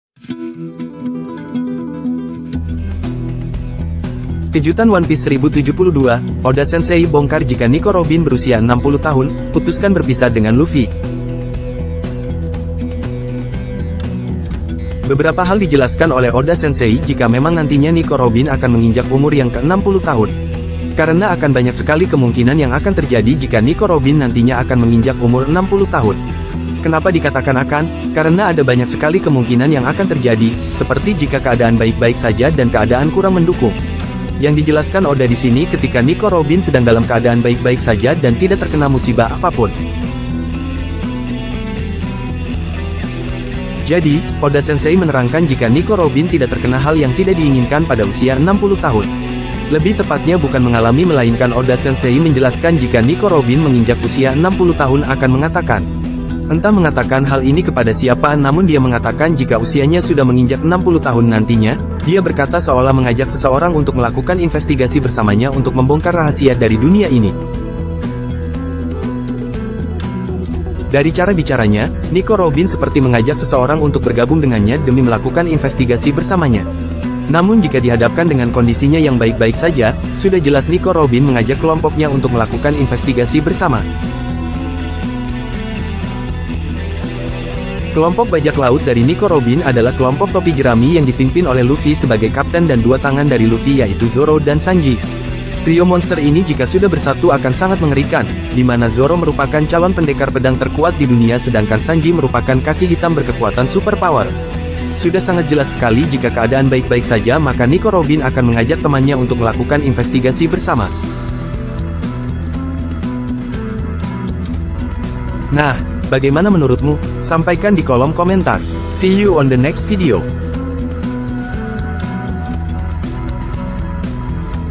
Kejutan One Piece 1072 Oda Sensei Bongkar Jika Nico Robin Berusia 60, Putuskan Berpisah dengan Luffy
Kejutan One Piece 1072: Oda Sensei Bongkar Jika Nico Robin Berusia 60 Tahun, Putuskan Berpisah dengan Luffy

Beberapa hal dijelaskan oleh Oda sensei jika memang nantinya Nico Robin akan menginjak umur yang ke 60 tahun. Karena akan banyak sekali kemungkinan yang akan terjadi jika Nico Robin nantinya akan menginjak umur 60 tahun. Kenapa dikatakan akan? Karena ada banyak sekali kemungkinan yang akan terjadi, seperti jika keadaan baik-baik saja dan keadaan kurang mendukung.
Yang dijelaskan Oda disini ketika Nico Robin sedang dalam keadaan baik-baik saja dan tidak terkena musibah apapun.

Jadi Oda sensei menerangkan jika Nico Robin tidak terkena hal yang tidak diinginkan pada usia 60 tahun akan mengalami.
Lebih tepatnya bukan mengalami melainkan Oda sensei menjelaskan jika Nico Robin menginjak usia 60 tahun akan mengatakan. Entah mengatakan hal ini kepada siapa namun dia mengatakan jika usianya sudah menginjak 60 tahun nantinya.Dia berkata seolah mengajak seseorang untuk melakukan investigasi bersamanya untuk membongkar rahasia dari dunia ini.

Dari cara bicaranya, Nico Robin seperti mengajak seseorang untuk bergabung dengannya demi melakukan investigasi bersamanya.
Namun jika dihadapkan dengan kondisinya yang baik-baik saja sudah jelas Nico Robin mengajak kelompoknya untuk melakukan investigasi bersama.

Kelompok bajak laut dari Nico Robin adalah kelompok topi jerami yang dipimpin oleh Luffy sebagai kapten dan 2 tangan dari Luffy yaitu Zoro dan Sanji. Trio monster ini jika sudah bersatu akan sangat mengerikan, dimana Zoro merupakan calon pendekar pedang terkuat di dunia sedangkan Sanji merupakan kaki hitam berkekuatan super power. Sudah sangat jelas sekali jika keadaan baik-baik saja maka Nico Robin akan mengajak temannya untuk melakukan investigasi bersama.

Dan yang paling penting, aktifkan tombol notifikasi agar selalu update dengan video terbaru